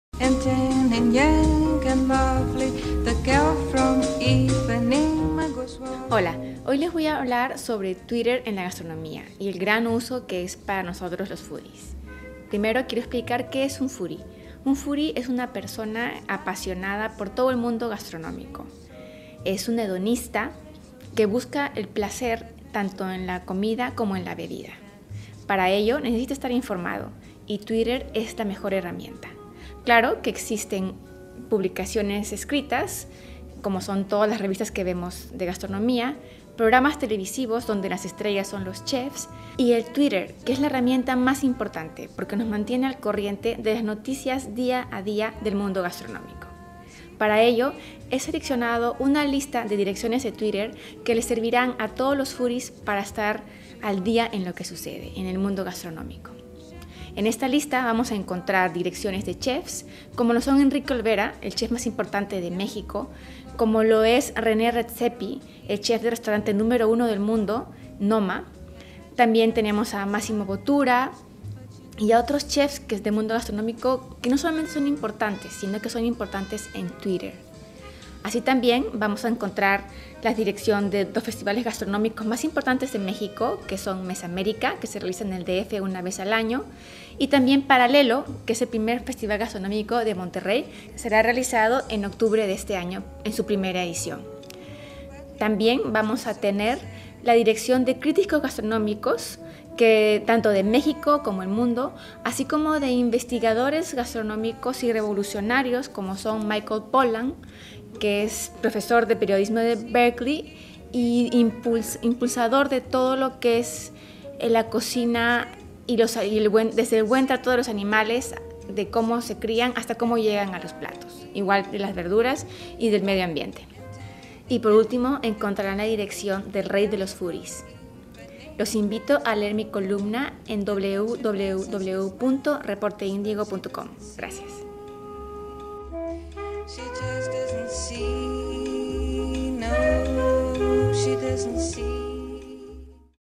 Reporte Indigo (Edición 573): Twitter y los ‘foodies’
Columna "Mía Gastronomía" de Karina Barbieri O'Hara.

En esta ocasión les comparto una lista de las cuentas de esta red social, que los foodies debemos tener para enriquecer nuestros conocimientos gastronómicos de manera nutritiva para la mente.